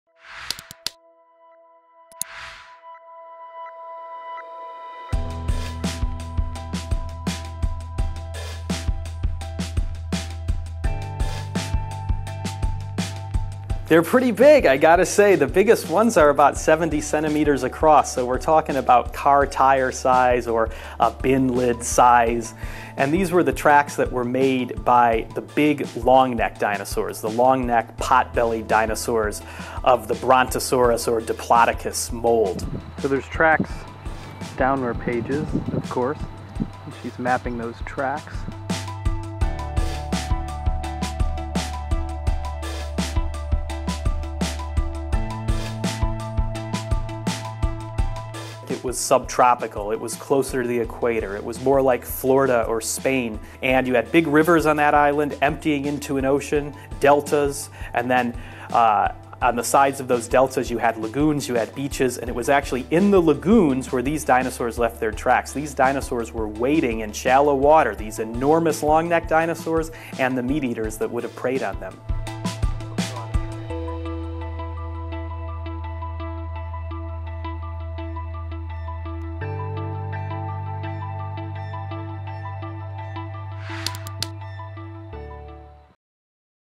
Sensationsfund in Schottland: 170 Millionen Jahre alte Saurierspuren entdeckt | DER SPIEGEL
Auf der Isle of Skye haben Forscher Dutzende Saurierspuren entdeckt. Die versteinerten Fußabdrücke stammen aus der Jurazeit und sind zum Teil so groß wie Autoreifen.